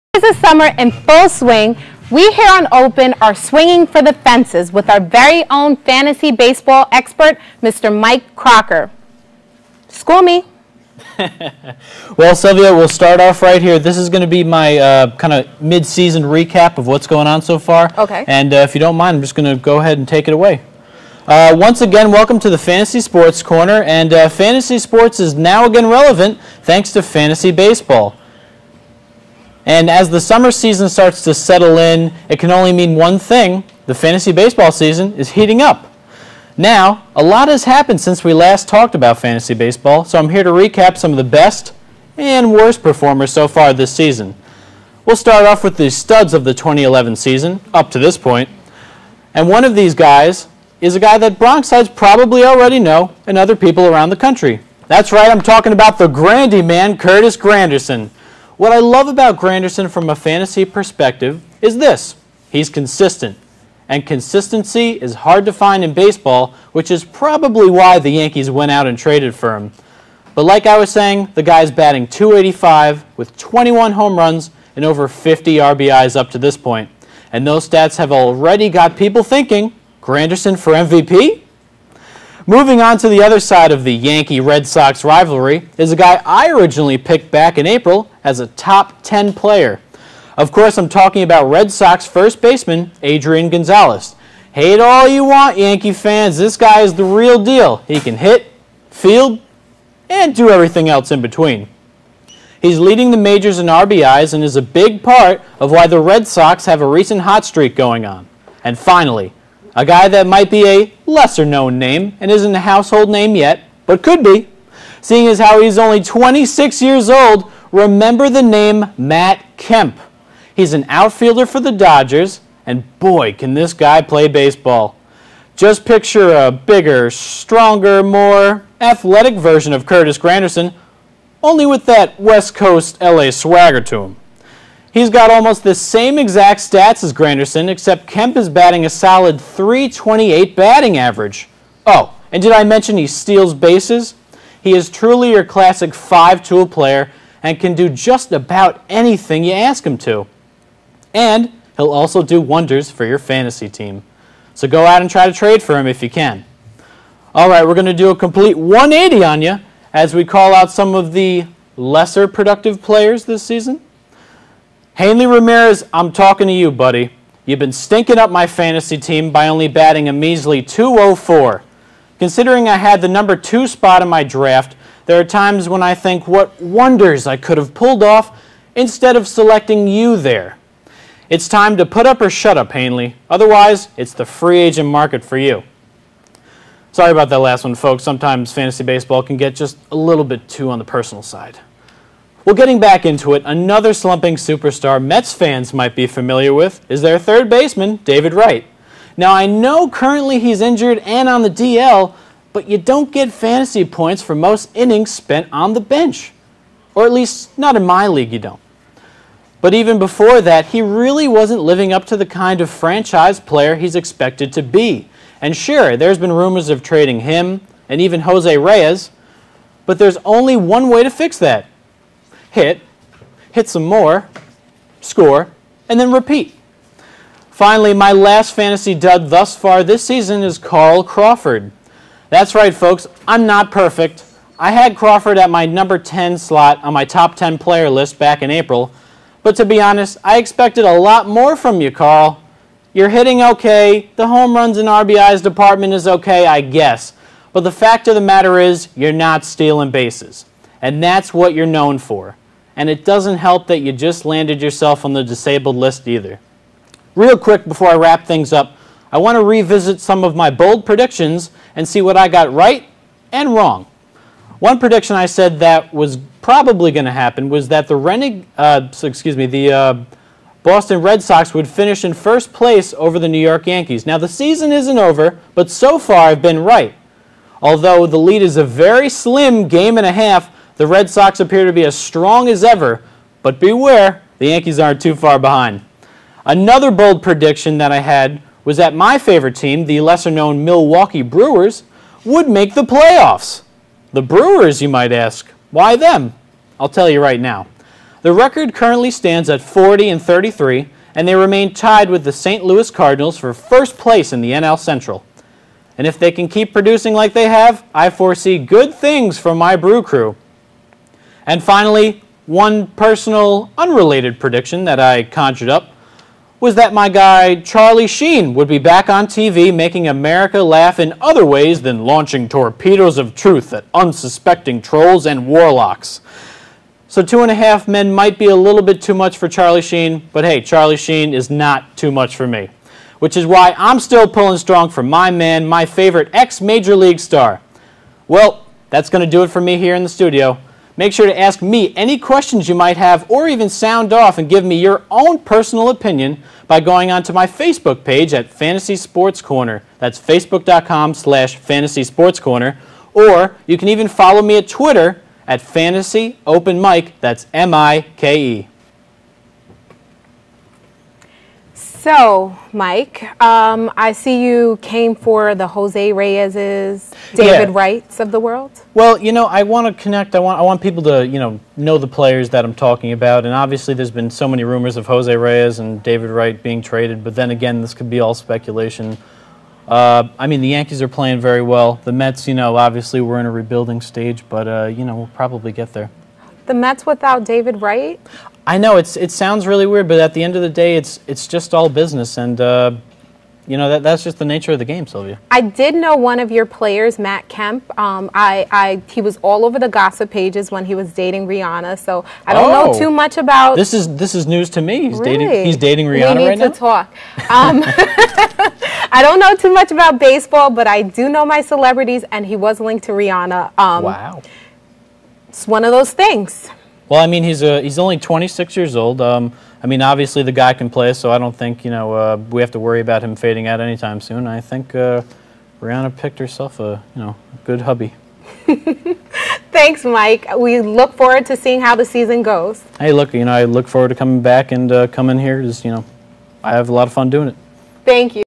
Mike Crocker Fantasy Baseball June 20th, 2011
The baseball season is heating up, and fantasy sports guru Mike Crocker takes his cuts on the OPEN baseball diamond, giving his take on the best, and worst from the season so far, and everything in between.

Got a fantasy baseball question for Mike?
and Mike will give you his expert advice!